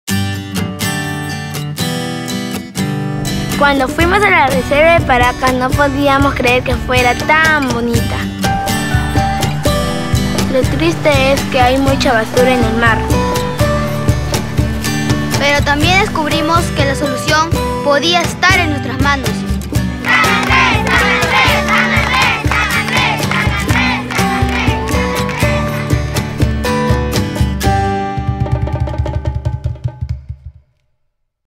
Los ángeles de San Andrés
El mar aguardaba por protección y un cambio que parecía imposible, hasta que un grupo de niños dio lo único que se necesitaba: voluntad.